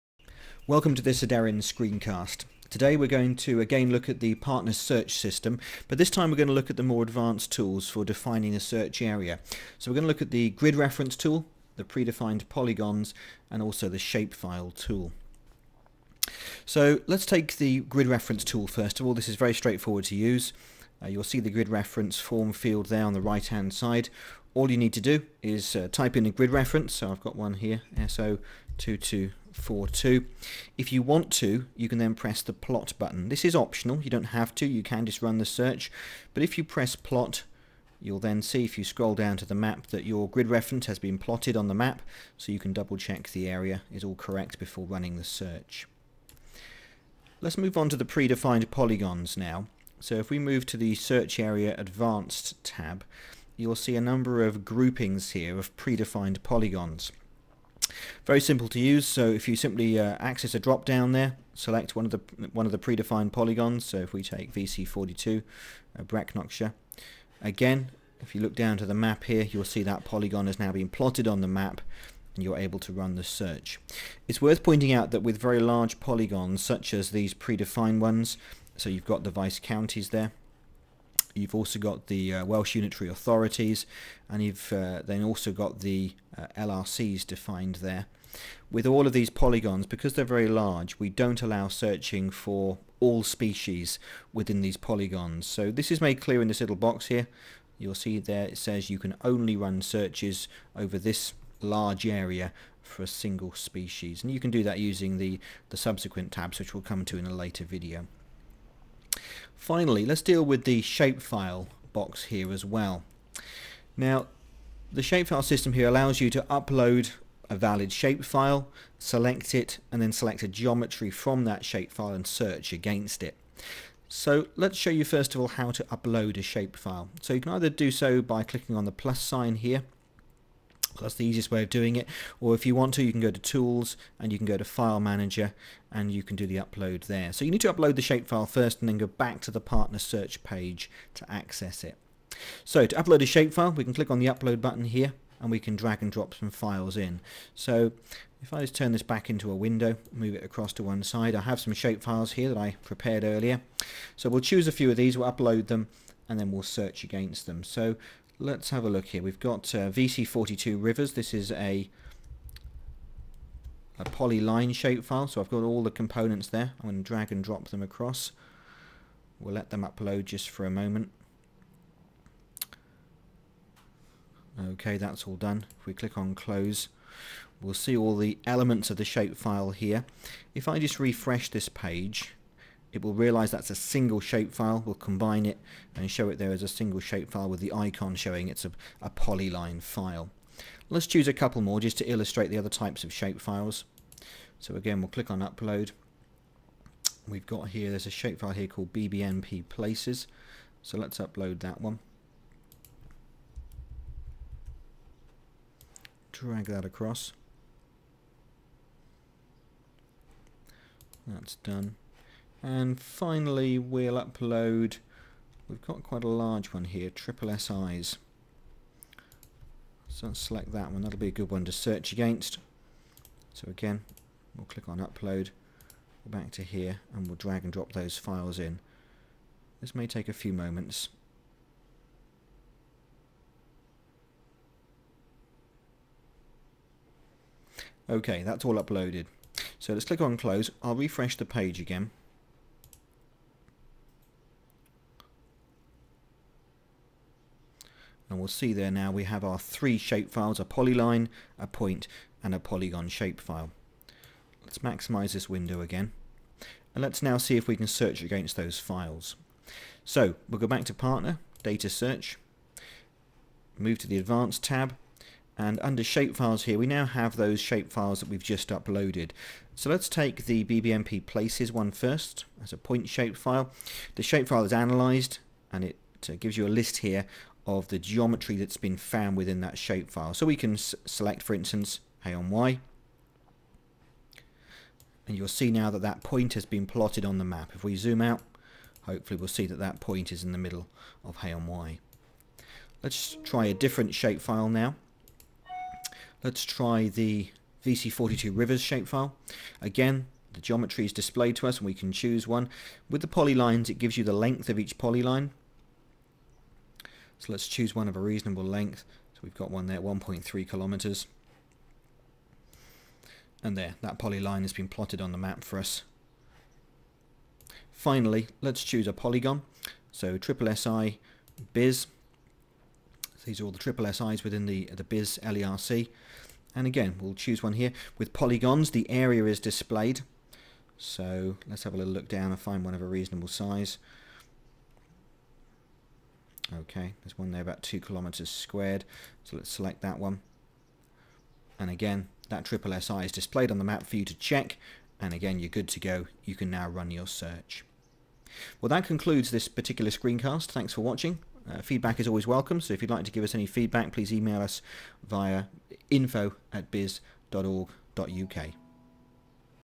Aderyn - Aderyn Partner Search Advanced Search Area
This screencast looks at the advanced tools available to define the search area including the grid reference tool, predefined polygons and the shapefile tool.